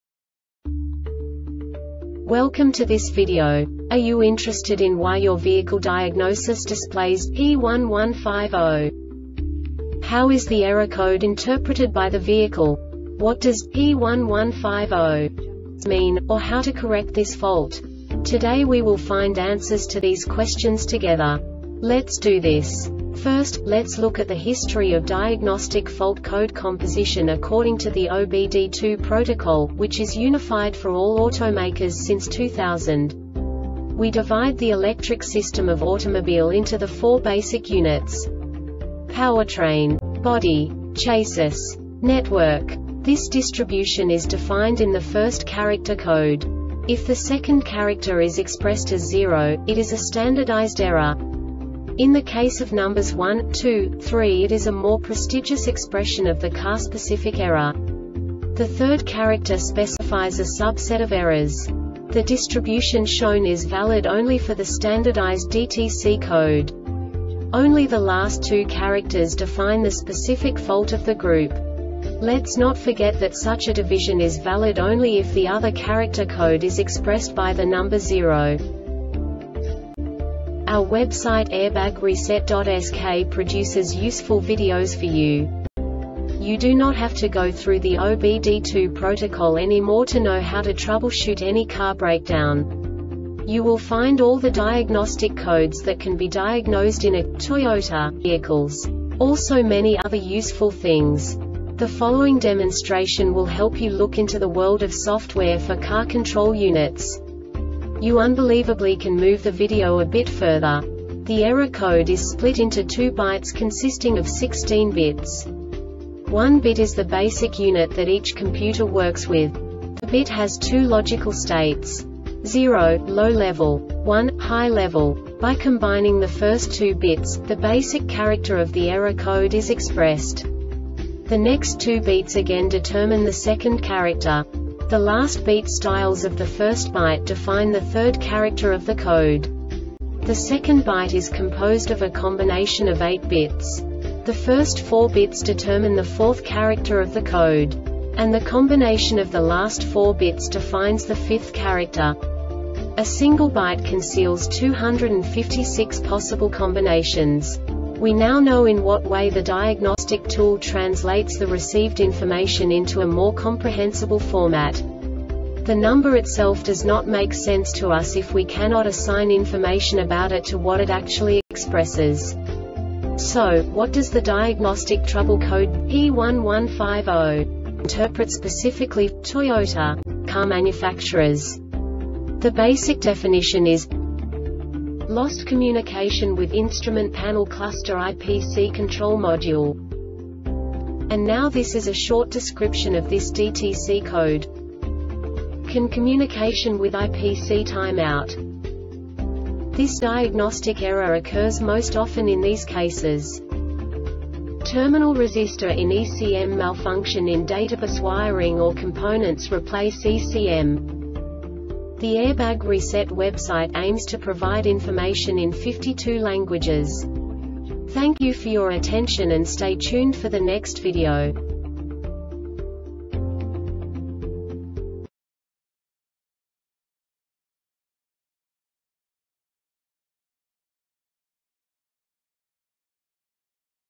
DTC Toyota P1150 Short Explanation
Contents:
0:21 Basic DTC analysis according to OBD2 protocol standard.
1:48 Insight into programming
2:58 A diagnostically understandable description of the Diagnostic Trouble Code
3:05 Basic error definition

Make:
Toyota
Code:
P1150
Definition:
Lost Communication With Instrument Panel Cluster (IPC) Control Module
Description:
CAN communication with IPC time out.